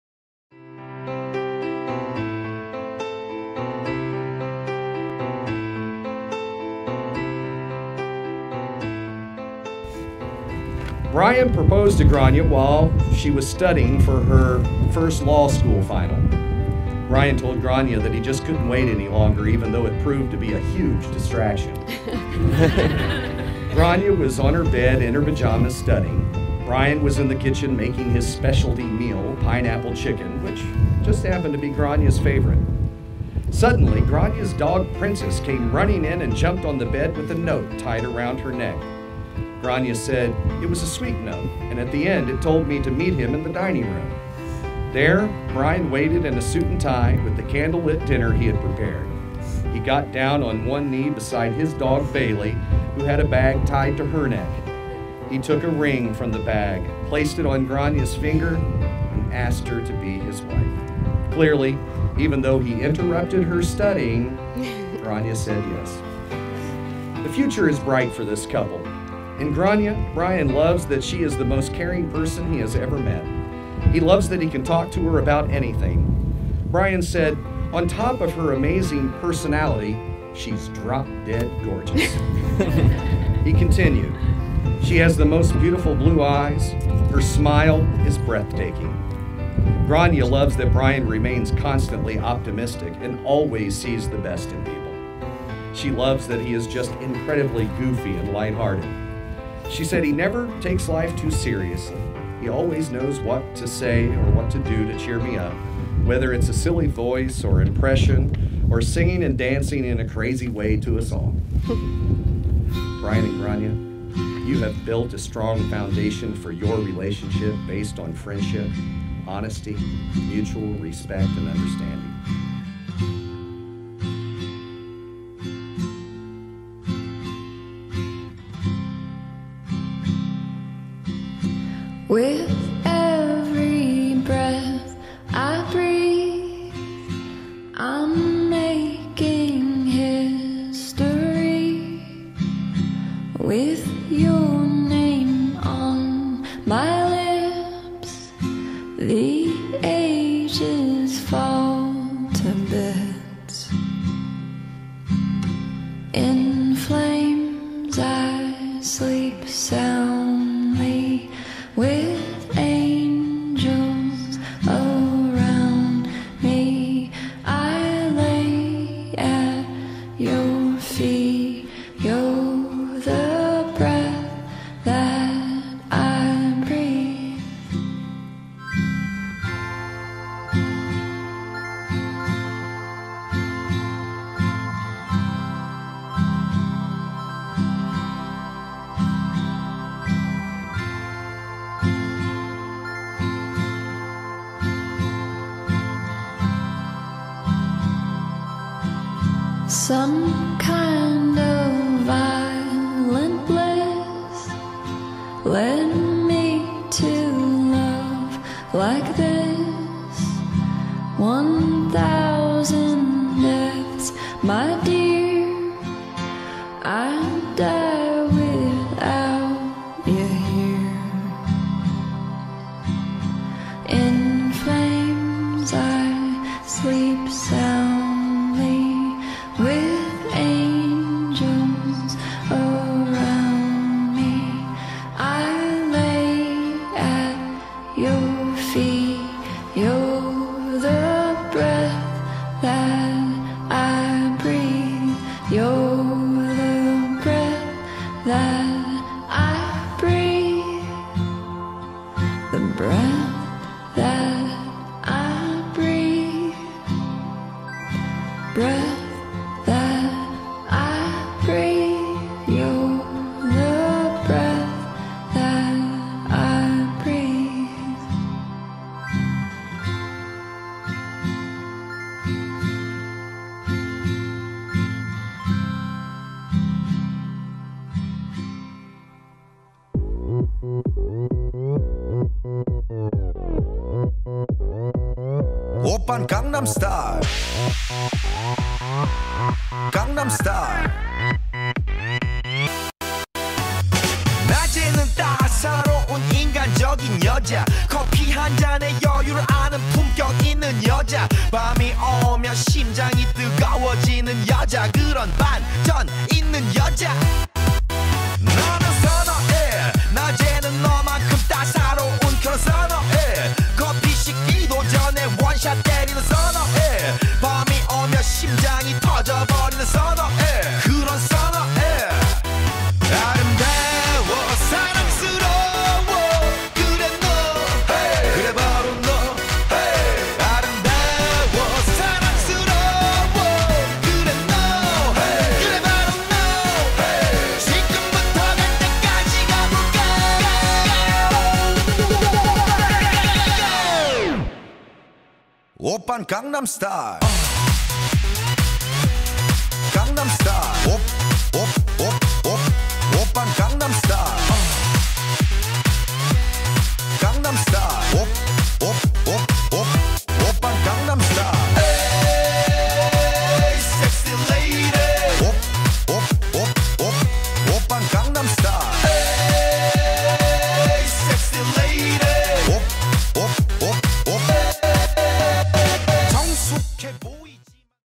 Bryan and Grainne Purgatory Golf Course Noblesville Indiana Wedding
Wedding Video for Bryan and Grainne's Wedding at Purgatory Golf Course in Noblesville Indiana by  Indianapolis Wedding Photographer RHatfield Photography